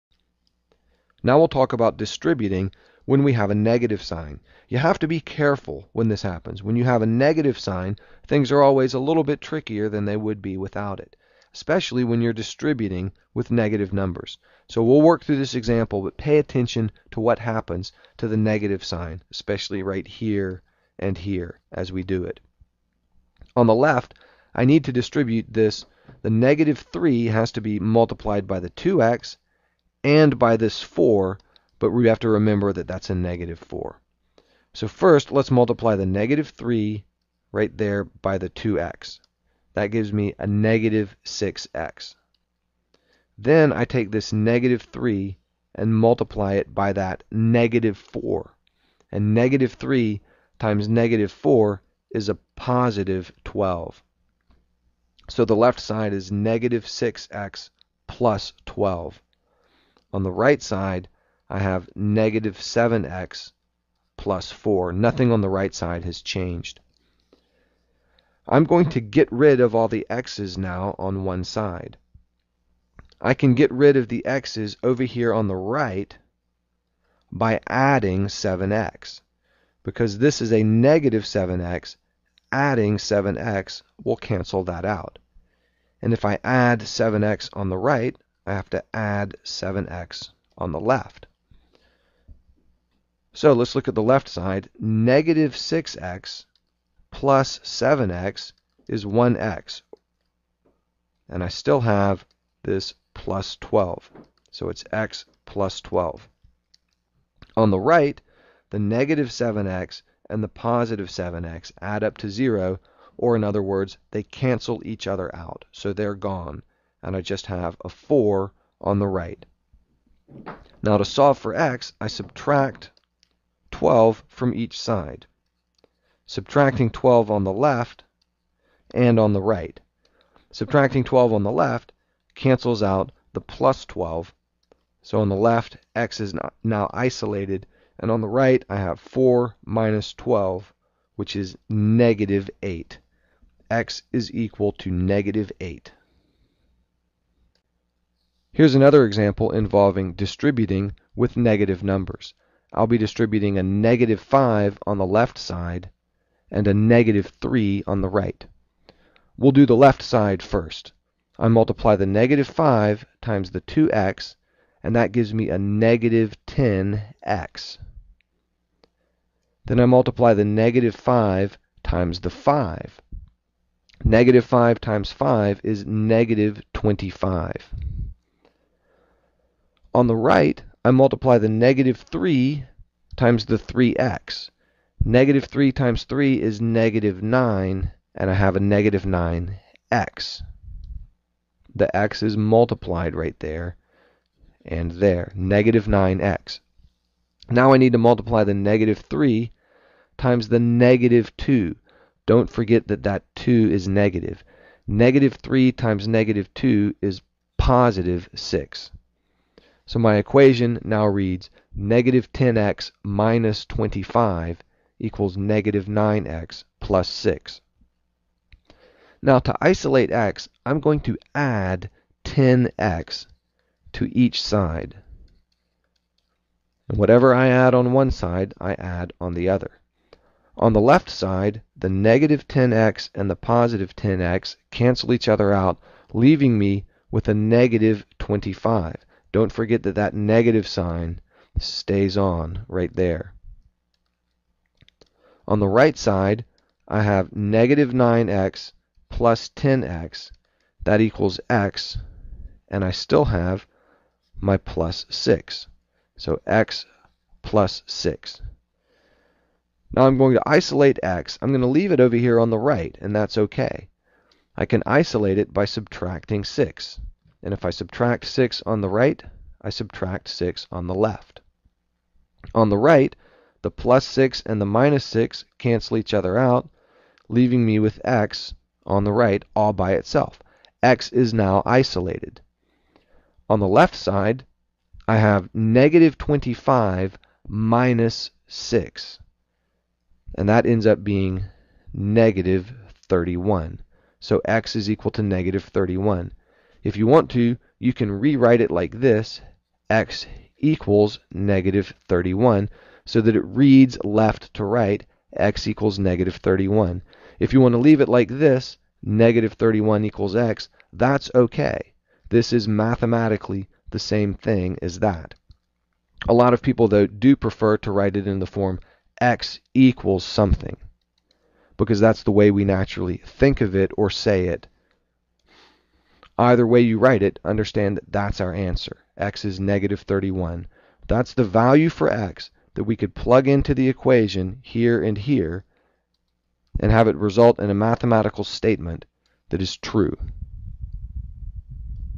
Prealgebra 2.10h Distributing a Negative Sign
Examples that involve distributing, combining like terms, and using the addition property, with attention given to the negative signs.  From the Prealgebra course by Derek Owens.